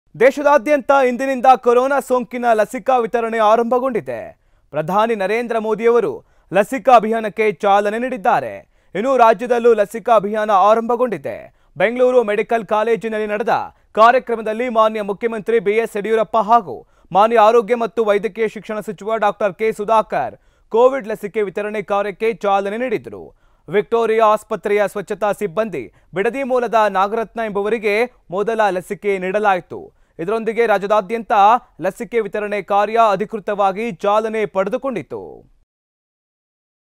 ರಾಜ್ಯದಲ್ಲಿ ಕೊರೊನಾ ಲಸಿಕಾ ಅಭಿಯಾನ ಆರಂಭ
ದೇಶಾದ್ಯಂತ ಇಂದಿನಿಂದ ಕೊರೋನಾ ಸೋಂಕಿನ ಲಸಿಕಾ ವಿತರಣೆ ಆರಂಭಗೊಂಡಿದೆ. ಪ್ರಧಾನಿ ನರೇಂದ್ರ ಮೋದಿಯವರು ಲಸಿಕಾ ಅಭಿಯಾನಕ್ಕೆ ಚಾಲನೆ ನೀಡಿದ್ದಾರೆ. ಇನ್ನೂ ರಾಜ್ಯದಲ್ಲೂ ಲಸಿಕಾ ಅಭಿಯಾನ ಆರಂಭಗೊಂಡಿದೆ. ಬೆಂಗಳೂರು ಮೆಡಿಕಲ್ ಕಾಲೇಜಿನಲ್ಲಿ ನಡೆದ ಕಾರ್ಯಕ್ರಮದಲ್ಲಿ ಮಾನ್ಯ ಮುಖ್ಯಮಂತ್ರಿ ಬಿ.ಎಸ್. ಯಡಿಯೂರಪ್ಪ ಹಾಗು ಮಾನ್ಯ ಆರೋಗ್ಯ ಮತ್ತು ವೈದ್ಯಕೀಯ ಶಿಕ್ಷಣ ಸಚಿವ ಡಾ. ಕೆ. ಸುಧಾಕರ್ ಕೋವಿಡ್ ಲಸಿಕೆ ವಿತರಣೆ ಕಾರ್ಯಕ್ಕೆ  ಚಾಲನೆ ನೀಡಿದರು. ವಿಕ್ಟೋರಿಯಾ ಆಸ್ಪತ್ರೆಯ ಸ್ವಚ್ಛತಾ ಸಿಬ್ಬಂದಿ, ಬಿಡದಿ ಮೂಲದ ನಾಗರತ್ನ ಎಂಬವರಿಗೆ ಮೊದಲ ಲಸಿಕೆ ನೀಡಲಾಯಿತು. ಇದರೊಂದಿಗೆ ರಾಜ್ಯಾದ್ಯಂತ ಲಸಿಕೆ ವಿತರಣೆ  ಕಾರ್ಯ  ಅಧಿಕೃತವಾಗಿ ಚಾಲನೆ ಪಡೆದುಕೊಂಡಿತು | Express TV Kannada